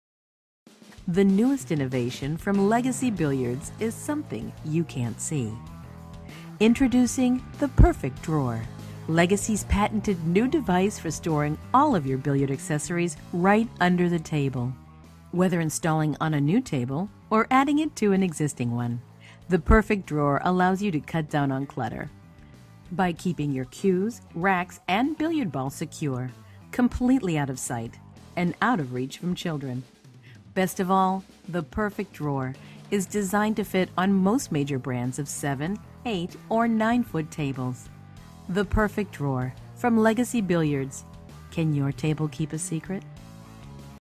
The Perfect Drawer
The first and only attachable billiard accessory storage drawer available, The Perfect Drawer, can be installed on most major brands of seven, eight and nine-foot pool and billiard tables. When not in use, The Perfect Drawer stores cues, chalk, billiard balls and other accessories safely and securely under the table and out of sight.